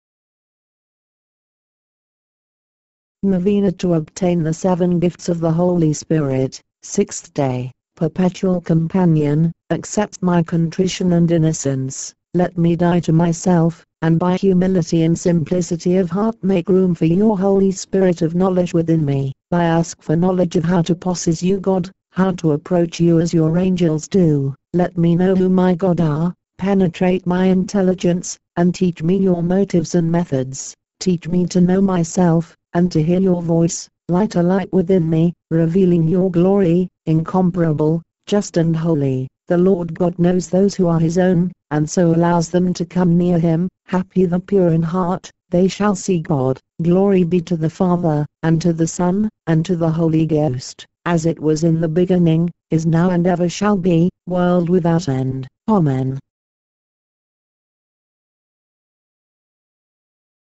NOVENA TO OBTAIN THE SEVEN GIFTS OF THE HOLY SPIRIT - Sixth Day
Perpetual Companion; accept my contrition and innocence. Let me die to myself and by humility and simplicity of heart make room for your Holy Spirit of Knowledge within me. I ask for Knowledge of how to posses you God; - how to approach you as your angels do; - let me know who my God are; - penetrate my intelligence and teach me your motives and methods. Teach me to know myself and to hear your voice. Light a light within me, revealing your Glory, Incomparable, Just and Holy.

"the Lord God knows those who are His Own and so allows them to come near Him;
happy the pure in heart: they shall see God;"

Glory be to the Father, and to the Son and to the Holy Ghost. As it was in the beginning, is now and ever shall be, world without end. Amen.